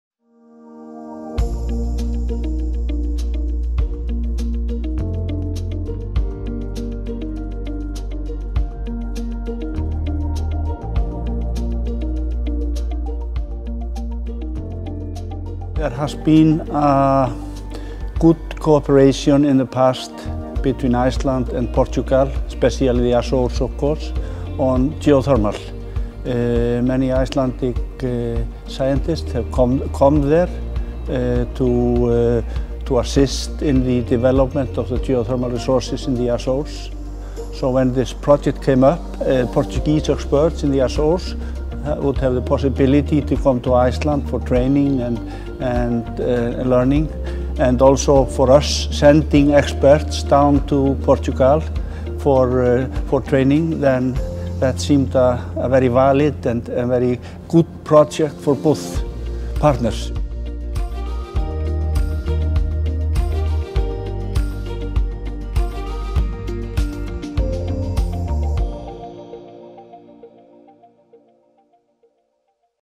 EEAchdropscount - Geothermal energy in the Azores: a sizzling collaboration with Iceland
Not long ago, 90% of the Azores’ energy was based on imported fossil fuels. That was not just bad news for the environment but also for the economy. With Iceland’s help, the Portuguese Archipelago started producing energy that’s green and local. How? Well, they tapped into one of the earth’s most explosive resources: magma!